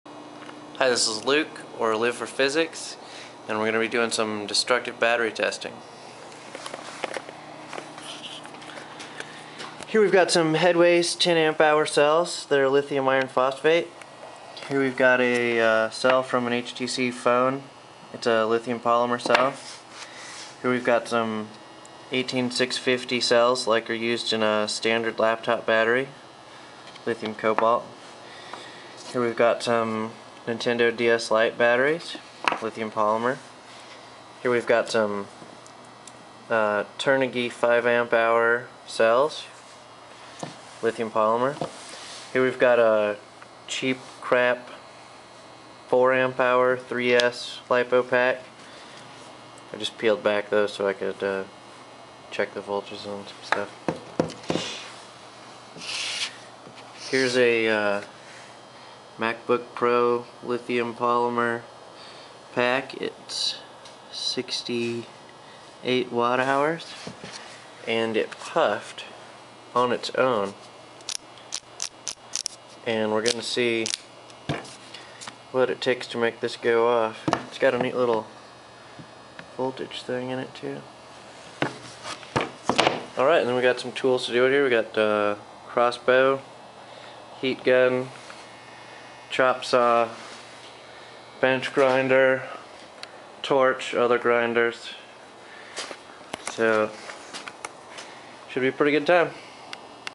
The intro to my cell testing videos
This is the intro that shows the various cells and tools to be used for the battery testing.